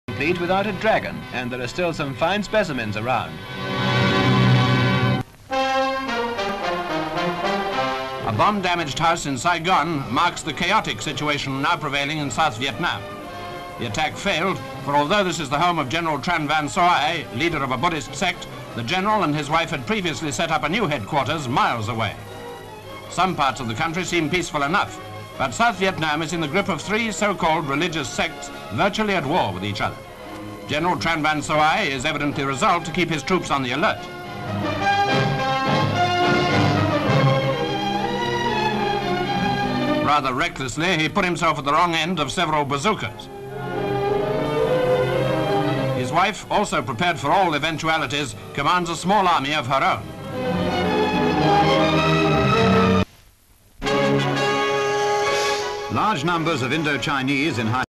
GENERAL TRAN VAN SOAI
(28 Apr 1955) South Vietnam is in the grip of three so-called "religious sects", virtually at war with each other.